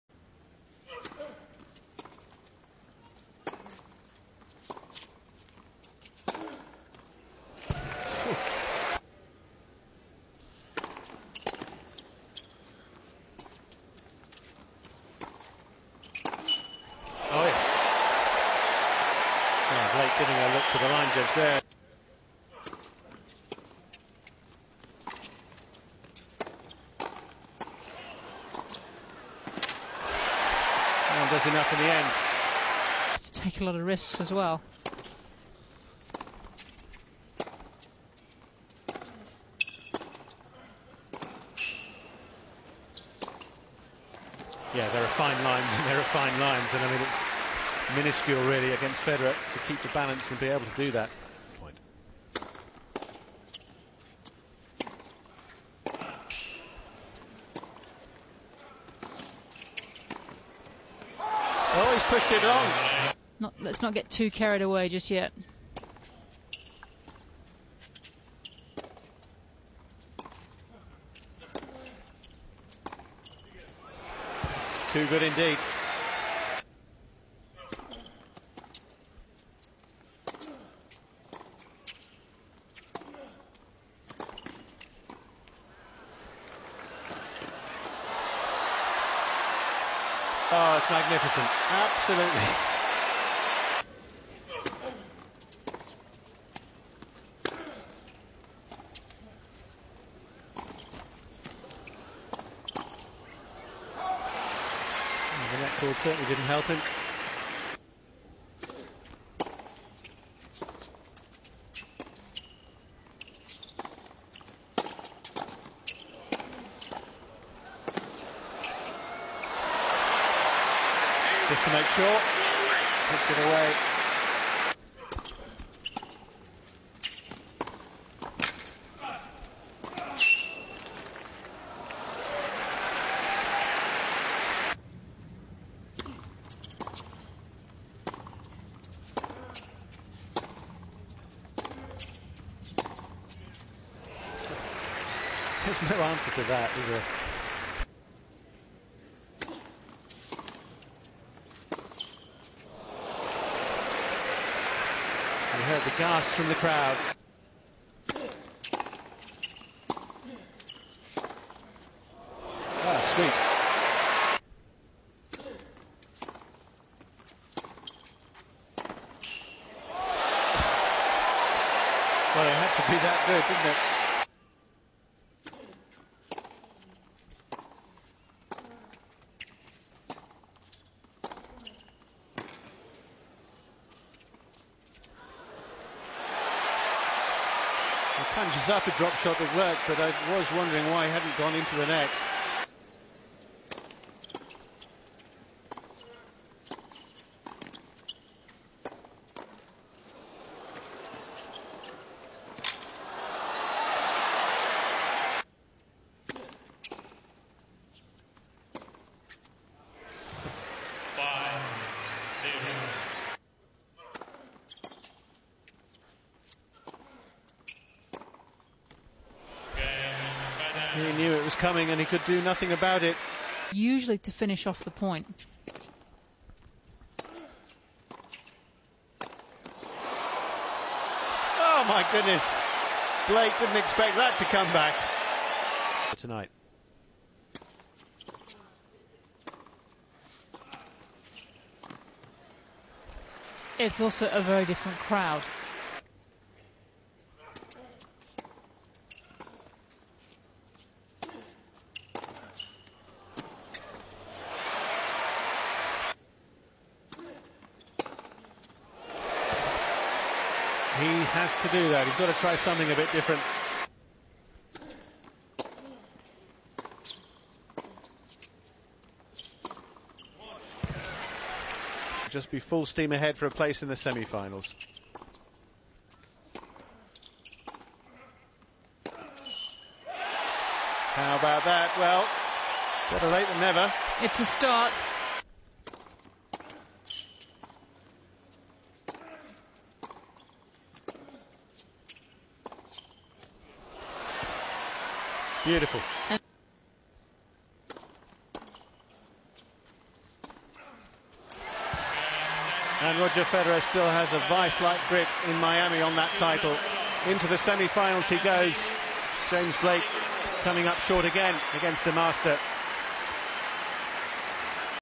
Federer vs Blake Miami 2006 QF highlights
Federer wins yet again over Blake